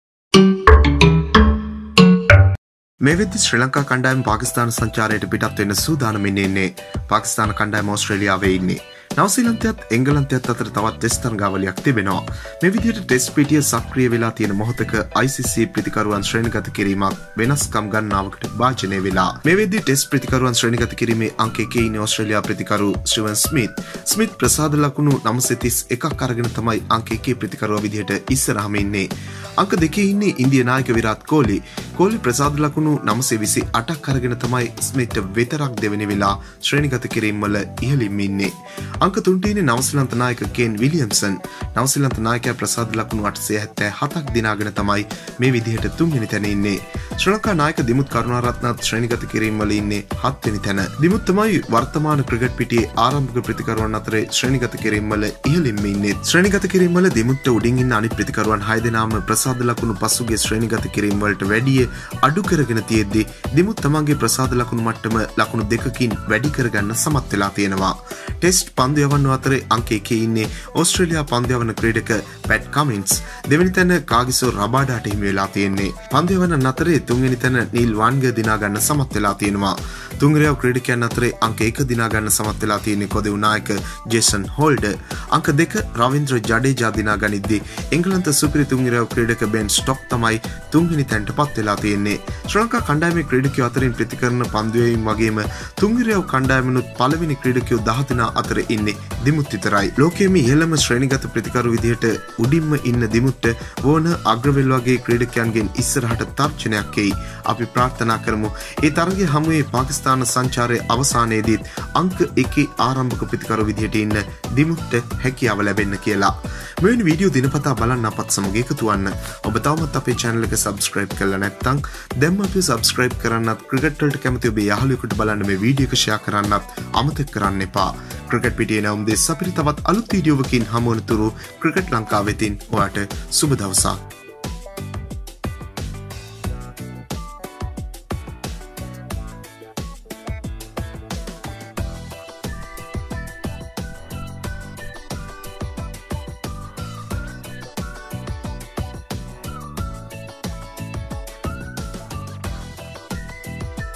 ආරම්භක පිතිකරුවන් අතරේ ලෝකයේම අංක එක දිමුත්ට
ශ්‍රී ලංකා කණ්ඩායමට පාකිස්ථාන සංචාරය ගොඩක් වැදගත් ,මේ වෙද්දී අපේ ක්‍රීඩකයෝ ශ්‍රේණිගත කිරීම් වල සතුටු දායක තැන් වල නෑ